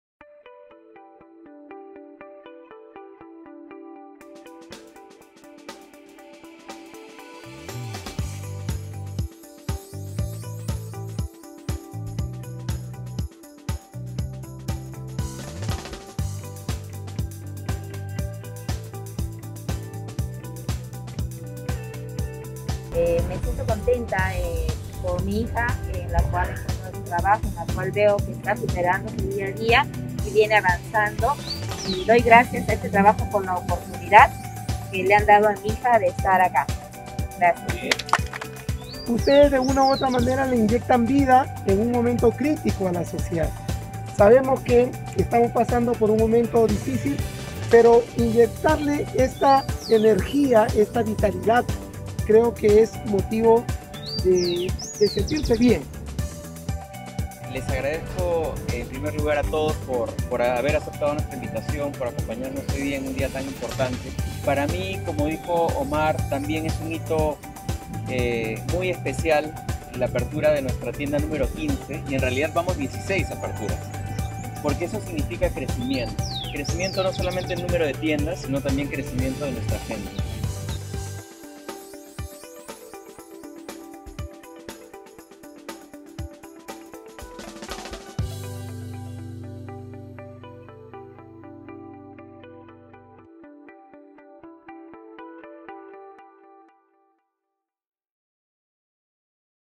¡Inauguramos nueva tienda en el MAC!
¿Ya conoces nuestra tienda en el Museo de Arte Contemporáneo? Visítanos y disfruta de un besomomento con nosotros. Tenemos ingreso libre, y además, ¡somos pet friendly!